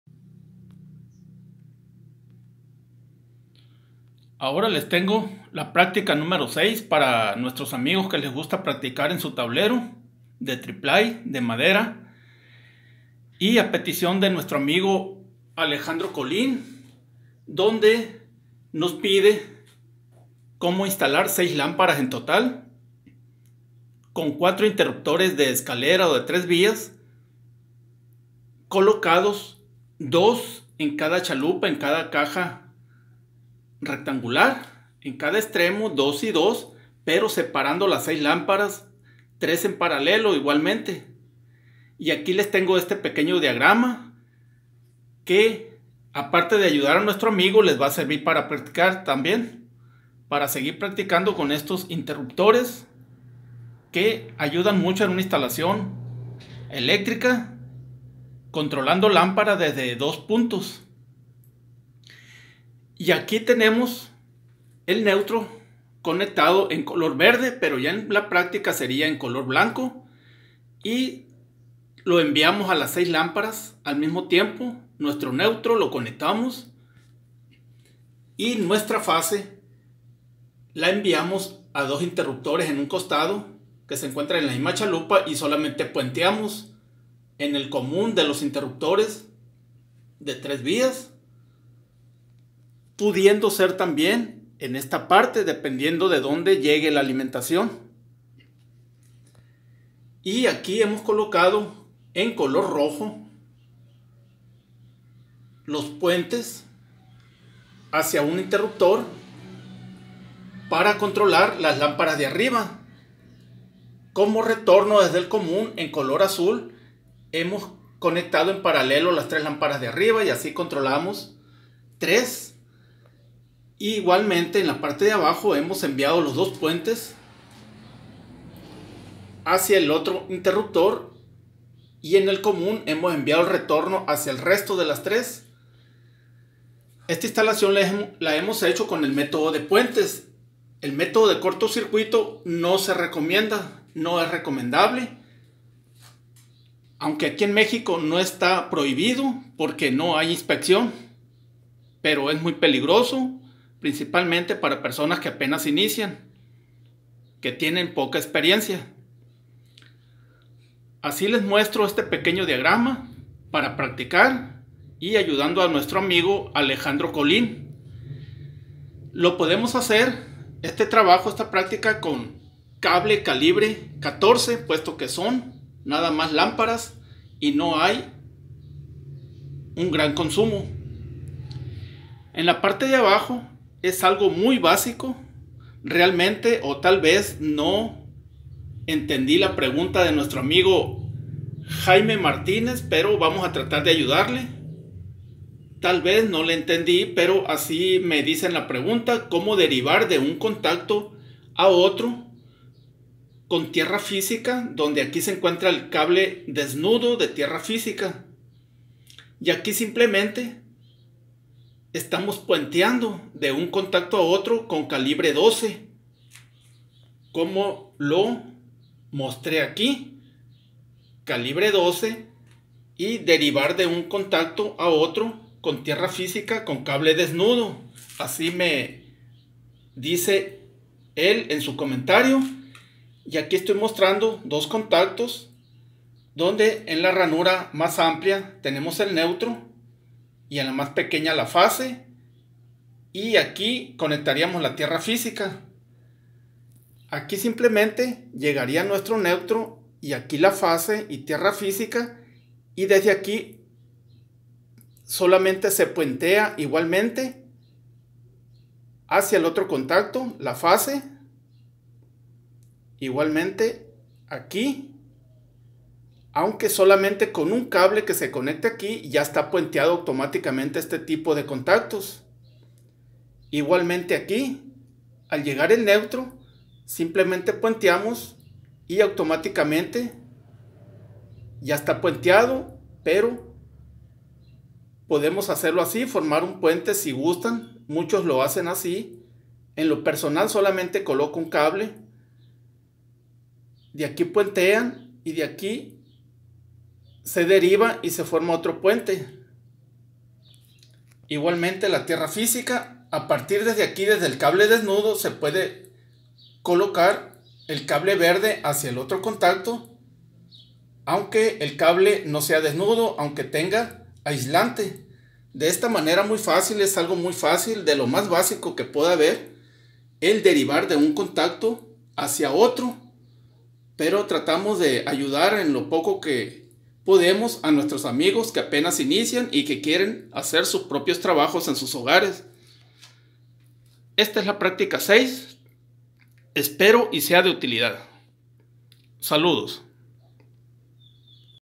PRÁCTICA N° 6, INST. 6 LÁMPS. EN TOTAL CON 4 INT. DE 3 VÍAS...DIVIDIENDO 3 Y 3 EN PARALELO...
Aquí seguimos con más prácticas y tratando de ayudar a nuestros amigos, como en este caso a nuestro amigo Alejandro Colín y Jaime Martínez, espero y sera de utilidad este nuevo y pequeño video, saludos...